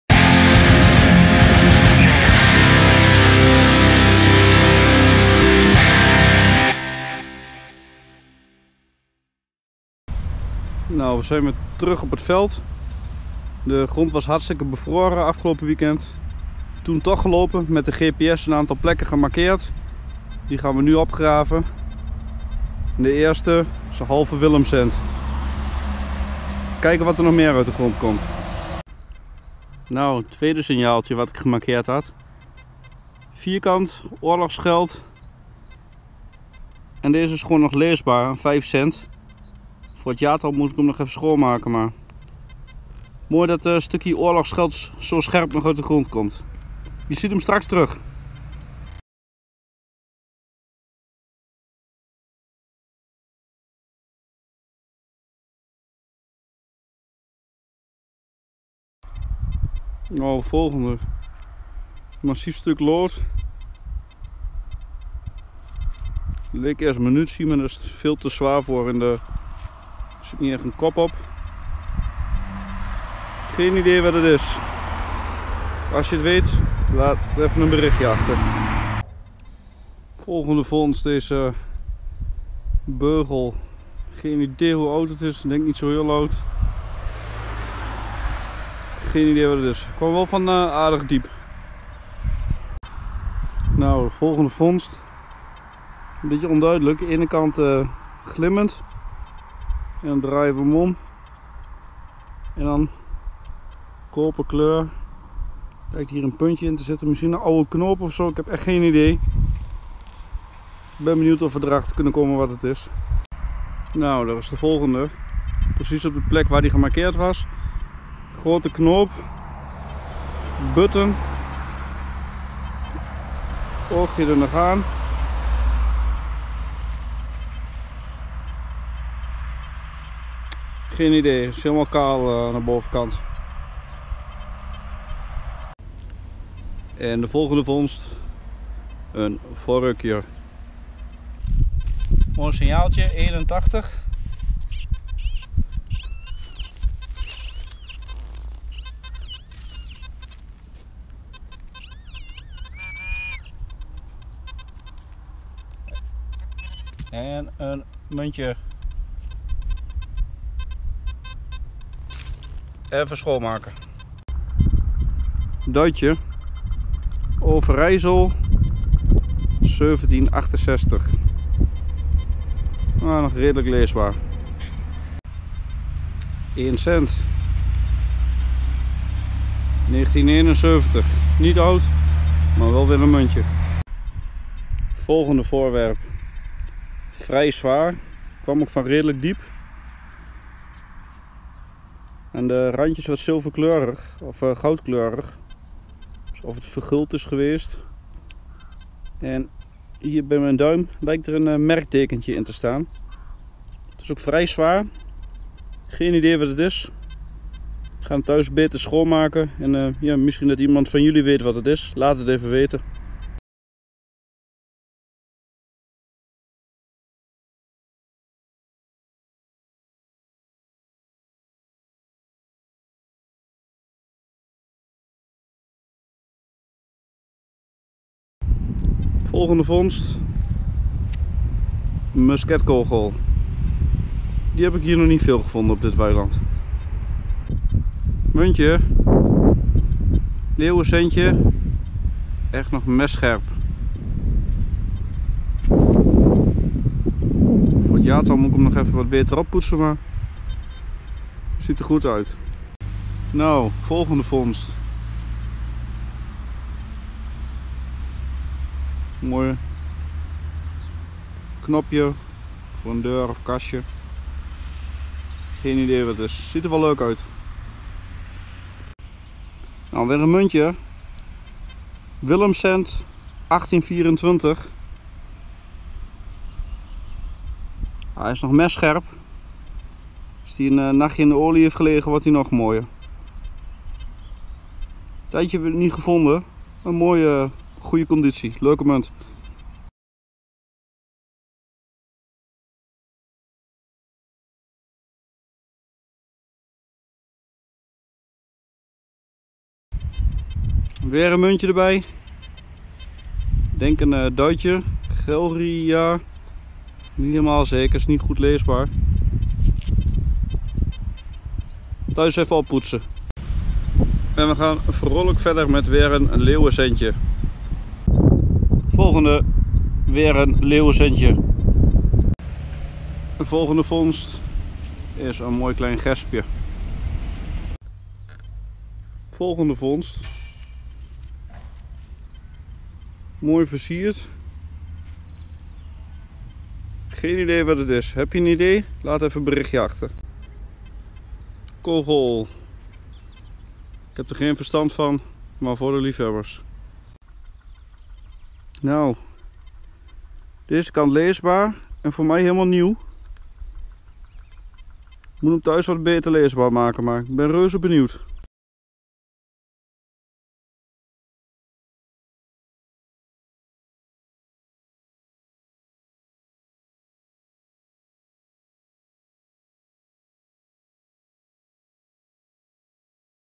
Trekkerbeugel van musket en munten met de XP Deus metaaldetector
Nog een paar vondsten van de laatste tijd van mijn favoriete weiland. Onder andere een trekkerbeugel van musket en een musket kogel. Verder zoals gebruikelijke enkele duiten, leeuwencenten, Willemcenten en een liard uit Luik uit 1750.